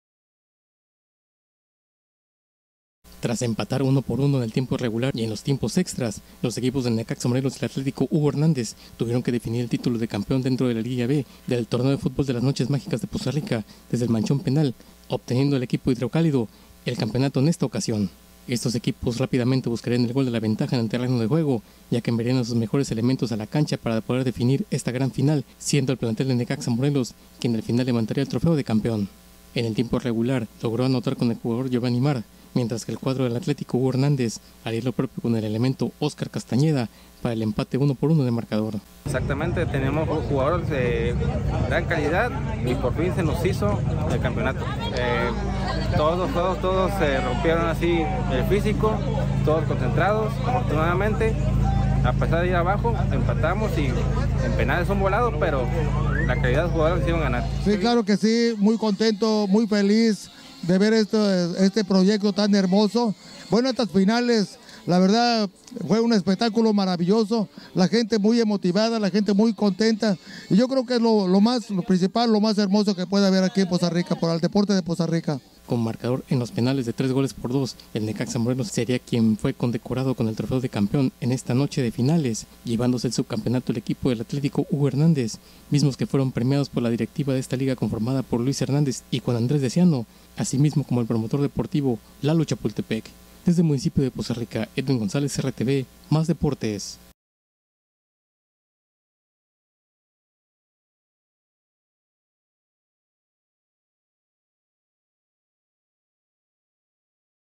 NECAXA MORELOS SE QUEDA CON EL CAMPEONATO
El equipo Necaxa  Morelos es campeón en la liguilla “B” del torneo de fútbol de las Noches Mágicas en Poza Rica.
Reportero: Edwin González